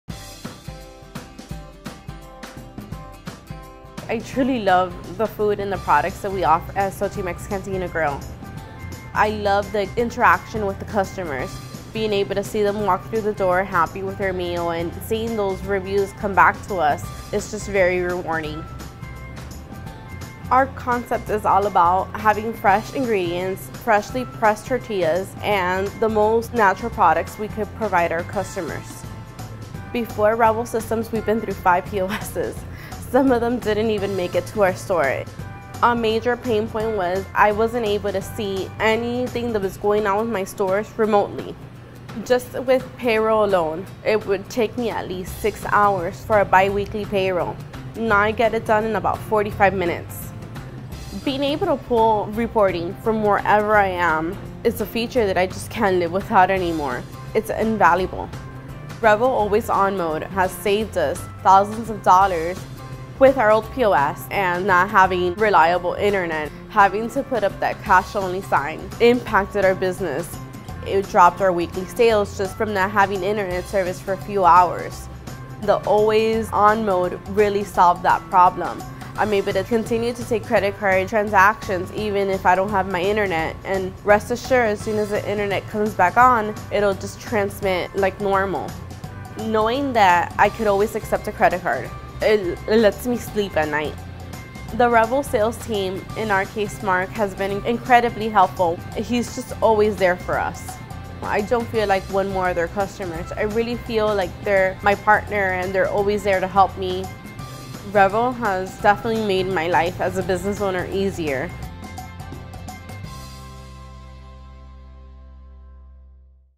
Xochimex Cantina Grill + Revel iPad Restaurant POS
Xochimex Cantina Grill is a quick service Mexican restaurant in Miami, Florida that's all about the freshest ingredients for their customers.

Xochimex is a fast growing franchise and with multiple locations, Revel iPad POS allows Rosemary, Co-Founder, to remotely check up on her stores' sales and labor.

Before Xochimex had Revel iPad POS, it would take Rosemary 6 hours to do a bi-weekly payroll.  With Revel, Rosemary does it in just 45 minutes.

Internet service is unpredictable, but with Revel Systems iPad POS' Always On Mode, Xochimex has saved thousands of dollars.

Rosemary loves working with the Revel Sales team and considers herself a partner instead of another customer.

"Revel has definitely made my life as a business owner easier." - Rosemary Diaz